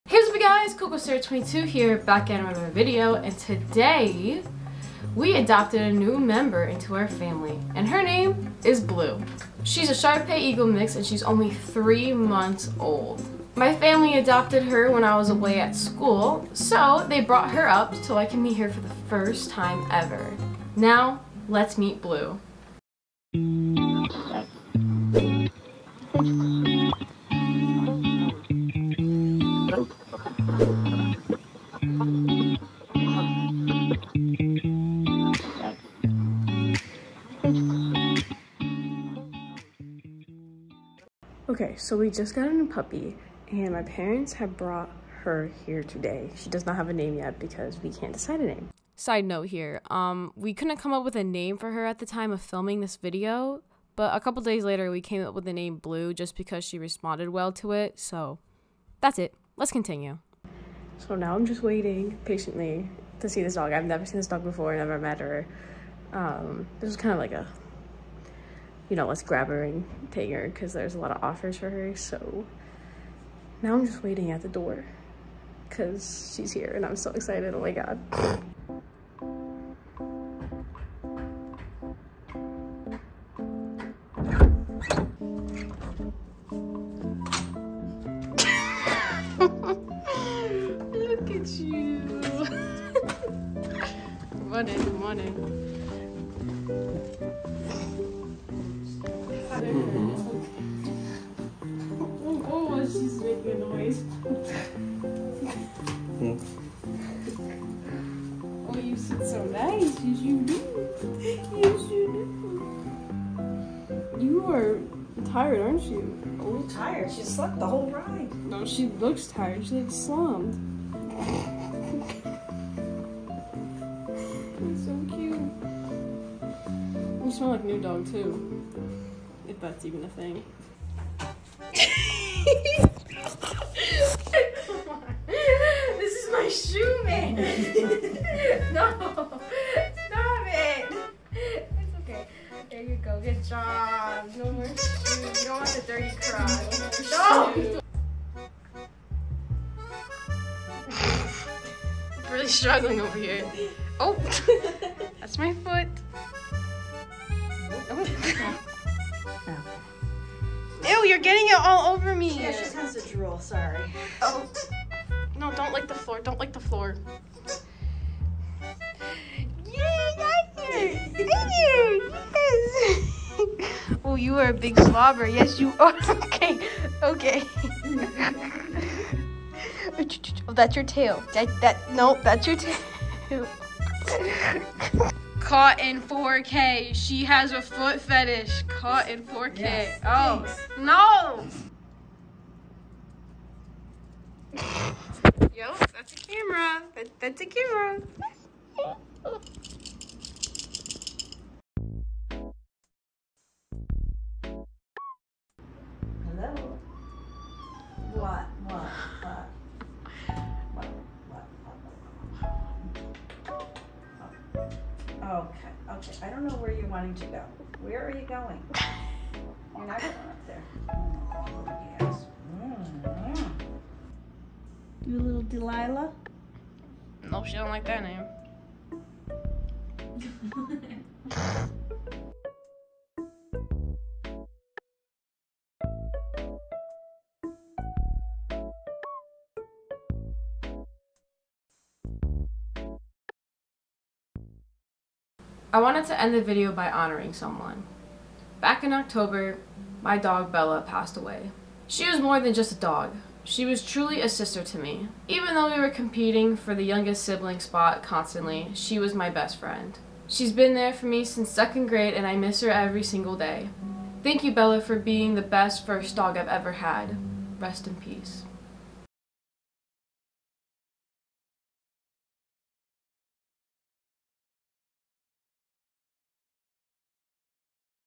WE GOT A PUPPY!!!
hey yall,

in this video, My family and I adopted a new puppy! watch to see meet Blu and learn more about her!